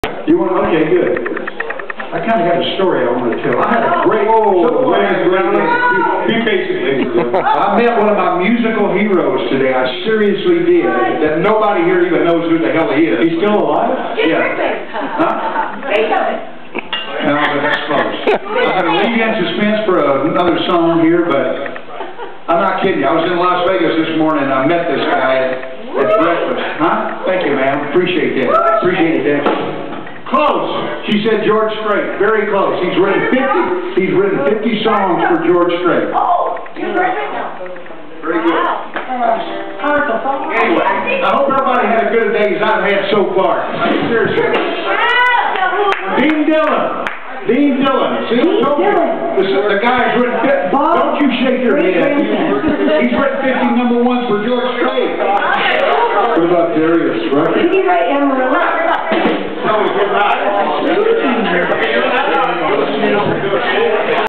100_0145-A bunch of chatting-Raucous @ Stocktons 2017-04-01
Just a bunch of chatting before a song.